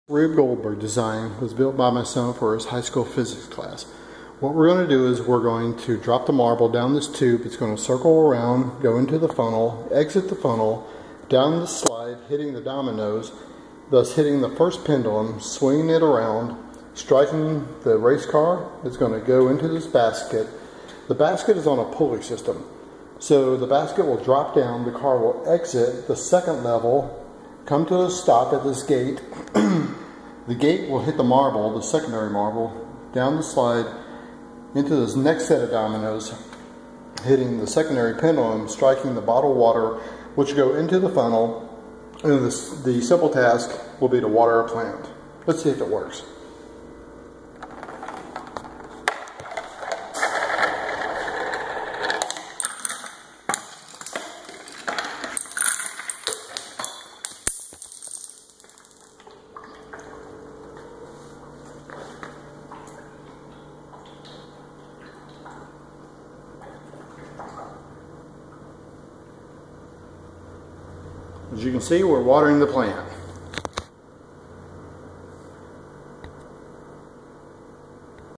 Simple Rube Goldberg Design
Rube Goldberg design using everyday items around the house.